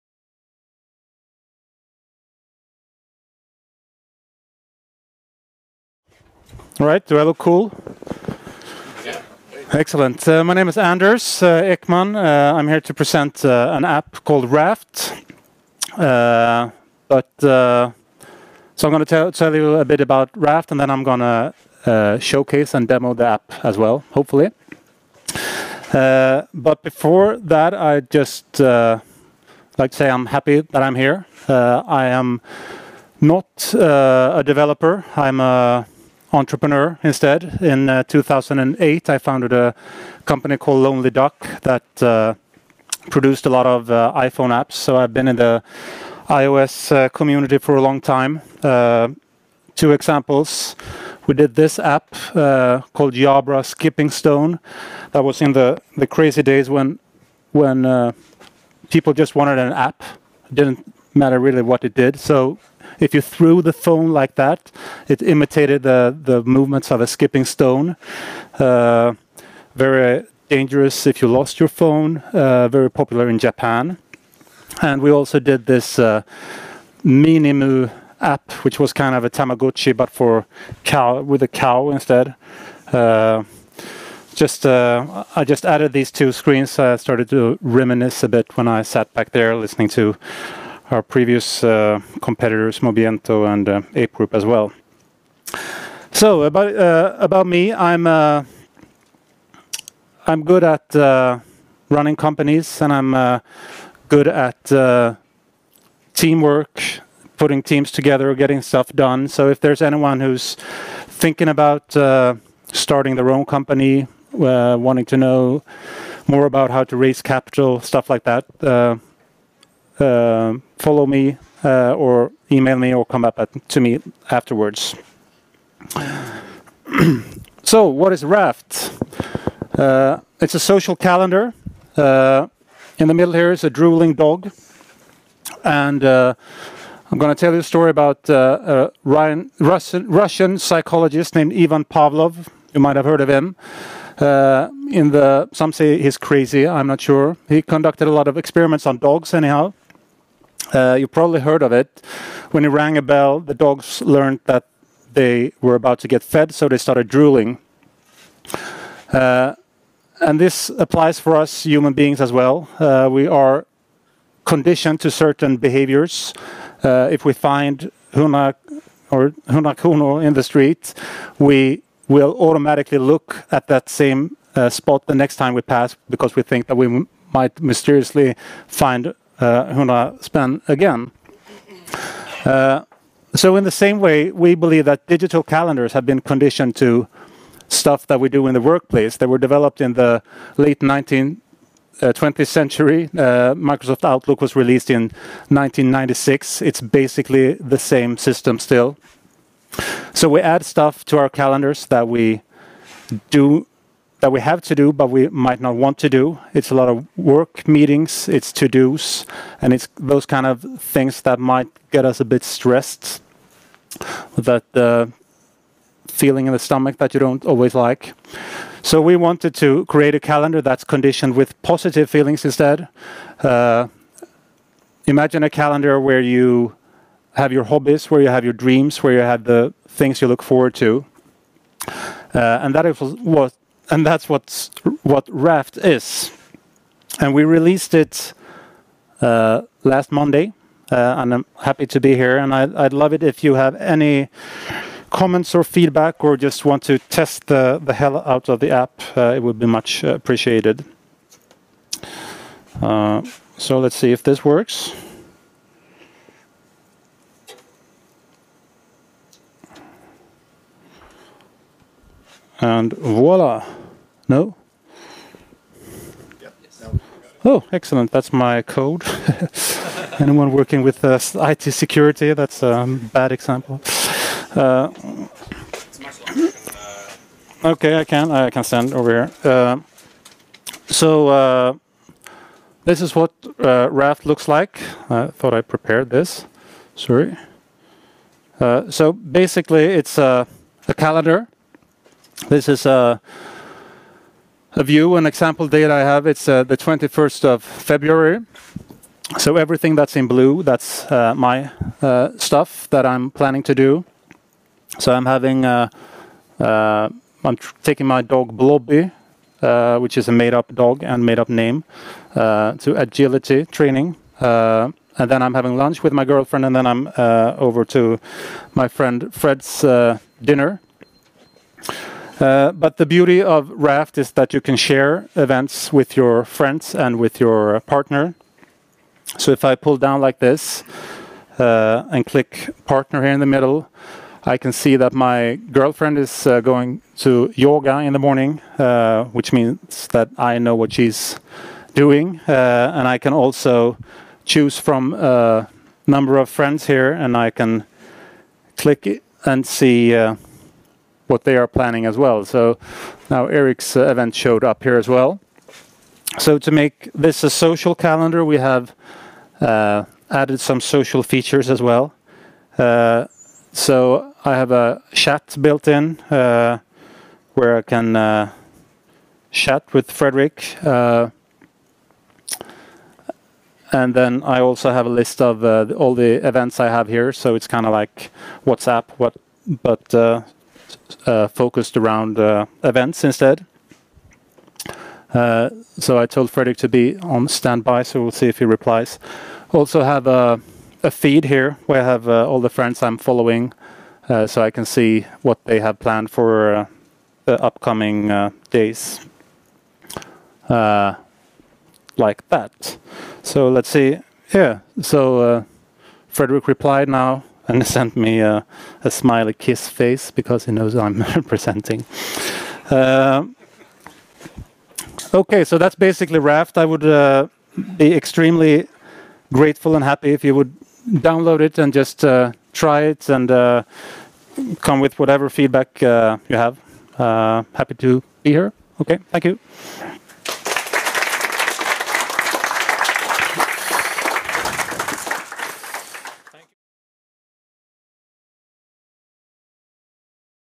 Raft (App presentation)
Recorded at CocoaHeads Stockholm hosted by Magento on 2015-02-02.